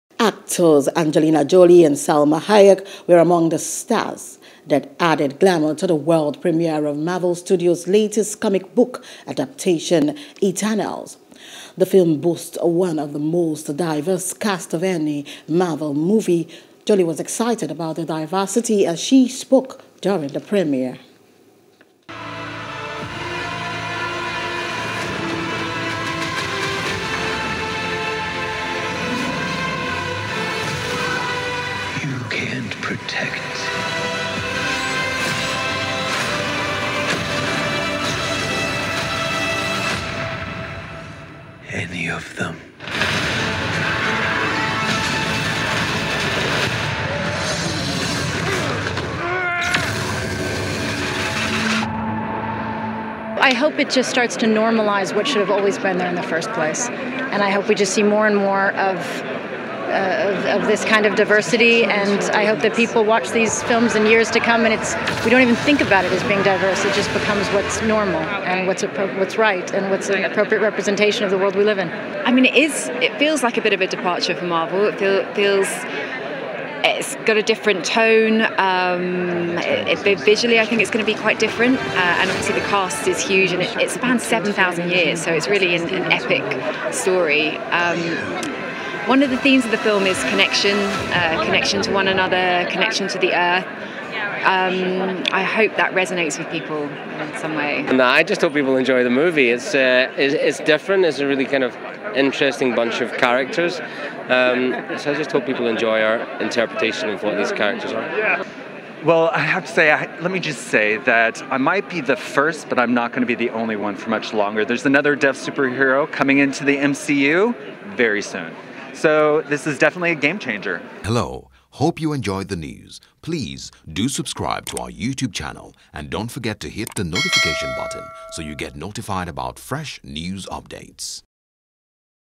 Stars Reunite For Premiere Of New Marvel Film | ENTERTAINMENT
Actors Angelina Jolie and Salma Hayek were among the stars that added glamor to the world premiere of Marvel Studios' latest comic book adaptation, 'Eternals'.